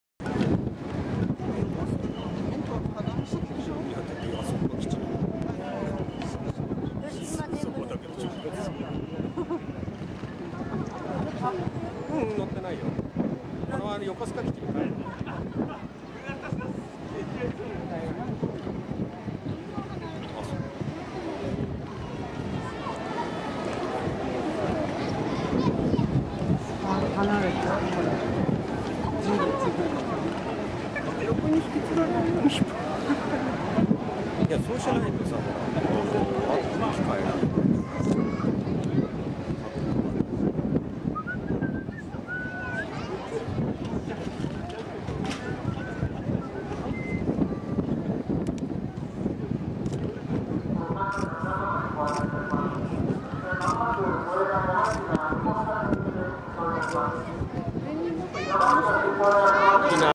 岸壁から離れる護衛艦やまゆき
２０１４年５月２５日撮影。
護衛艦やまゆきが船橋港から出航する様子。砕氷船の５階部分から撮影したので珍しいです。